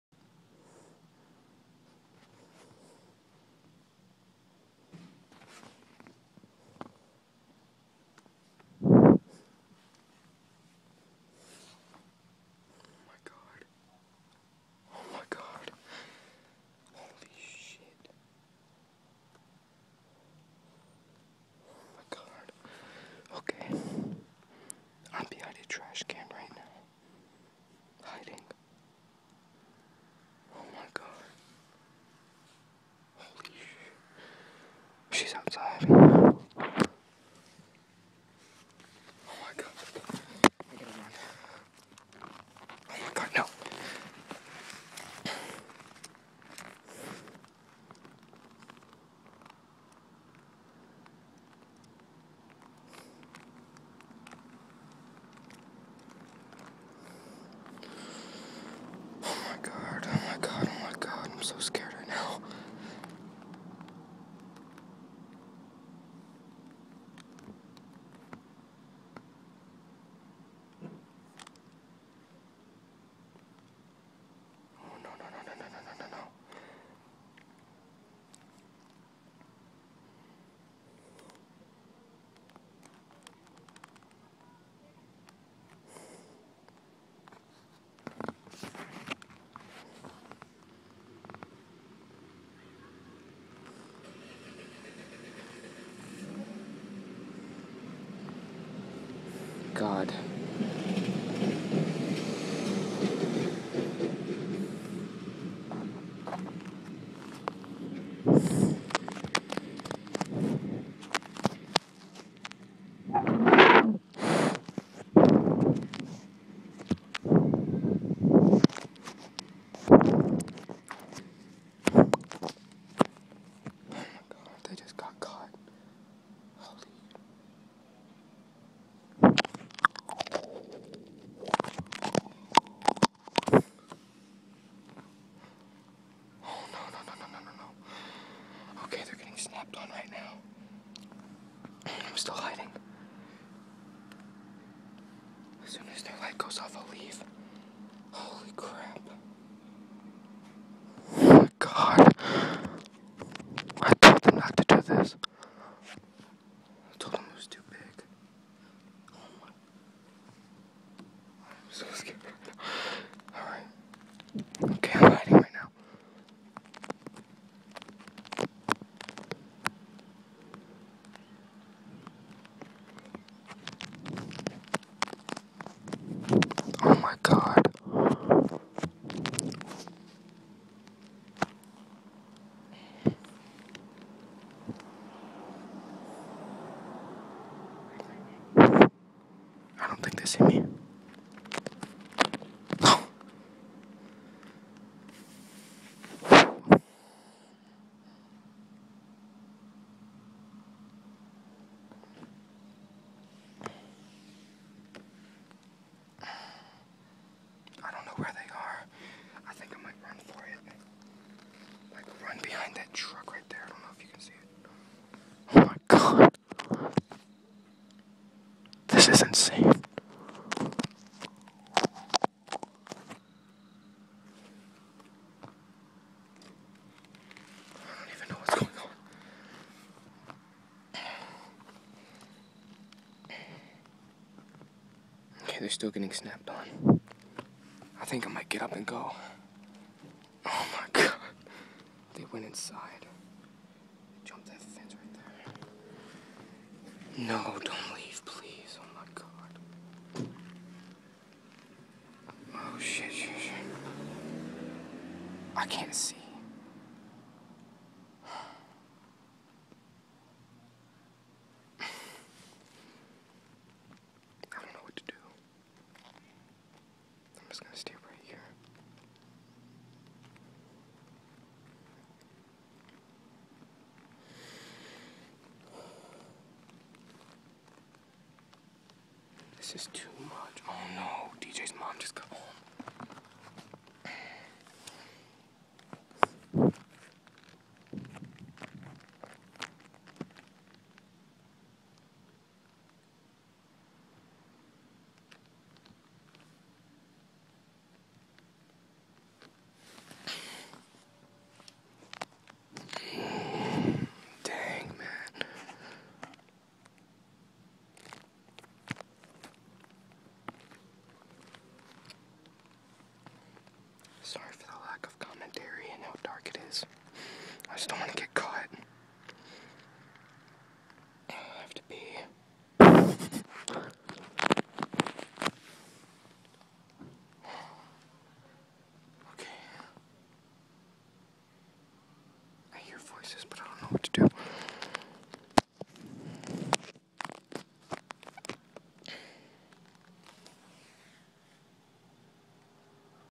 Retards: THE BREAK IN part 1
After figuring out that out friends back door was open, my two friends went in. While I was keeping guard outside, someone in the house woke up. The mom.